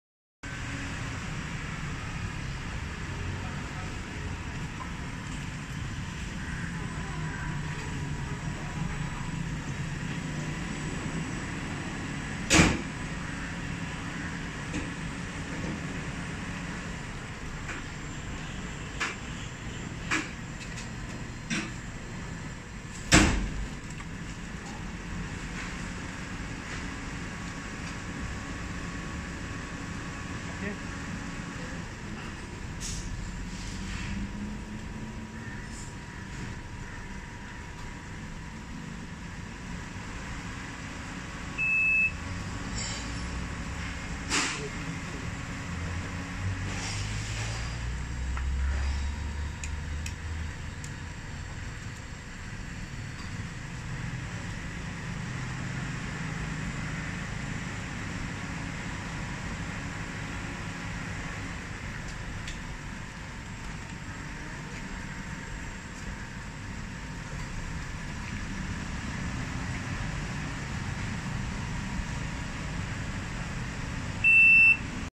KODO Welcab Platinum - Toyota Sienta - Car Seat for Disabled OKU
KODO Welcab Platinum

• The passenger seat is an electrically operated detachable lift-up and
        swing out that can be used as a wheelchair.
• It allows problem-free access and alighting with help of an electrically
        operated lift.
• All genuine leather cover
• Custom made leather colour to match your existing seat colour
• Adjustable armrest
• Removable feet rest
• All in one solution
• Back wheel disk brake
• Stationary safety lock
• 180 degree backrest adjustability
• Comfortable handmade cushion
• Loading Capacity : 120kg